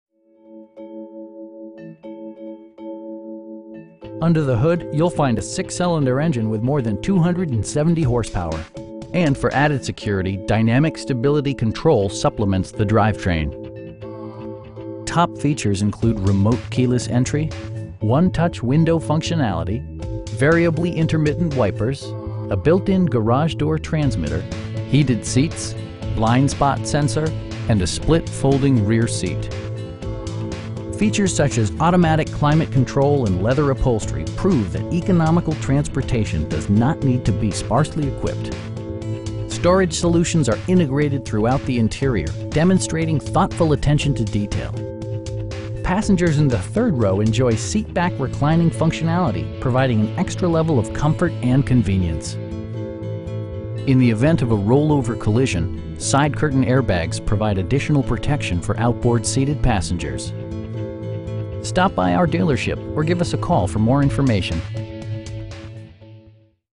2017 Chrysler Pacifica Touring-L in Wilson, NC 27896
Lee Chrysler Dodge Jeep Ram

Under the hood you'll find a 6 cylinder engine with more than 270 horsepower, and for added security, dynamic Stability Control supplements the drivetrain. Top features include remote keyless entry, 1-touch window functionality, variably intermittent wipers, a built-in garage door transmitter, heated seats, tilt steering wheel, blind spot sensor, and a split folding rear seat. Features such as automatic climate control and leather upholstery prove that economical transportation does not need to be sparsely equipped. Storage solutions are integrated throughout the interior, demonstrating thoughtful attention to detail. Passengers in the third row enjoy seat back reclining functionality, providing an extra level of comfort and convenience. In the event of a rollover collision, side curtain airbags provide additional protection for outboard seated passengers. We have the vehicle you've been searching for at a price you can afford. Stop by our dealership or give us a call for more information.